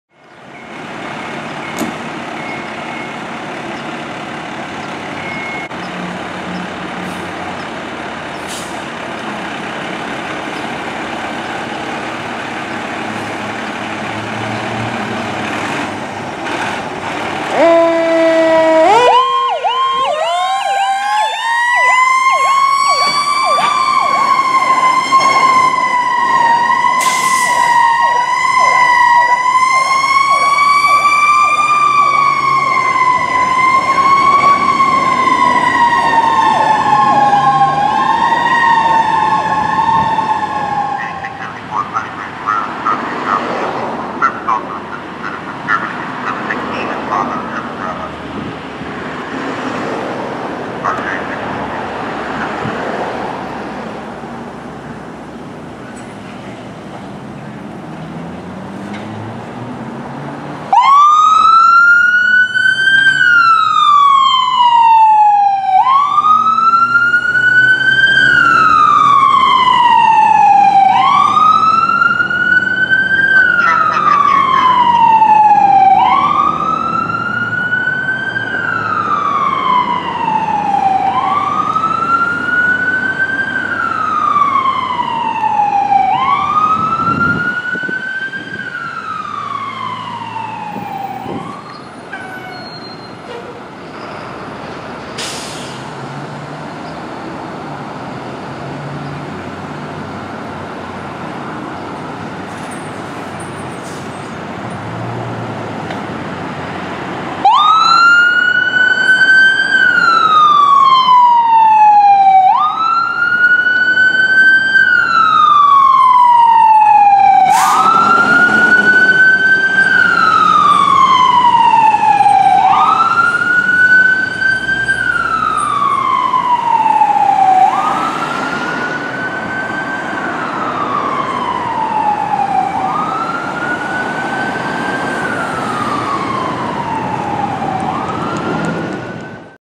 Montgomery County Rescue Squad 742, Ambulance 742, and Ambulance 742C Responding
Montgomery County Fire and Rescue Service (Wheaton Volunteer Rescue Squad) Rescue Squad 742, Ambulance 742, and Ambulance 742 Charlie all responding to separate assignments.

Unit(s) Seen Responding:
Rescue Squad 742 is a 2016 Seagrave Marauder ll.

Montgomery County, MD - (8/2018).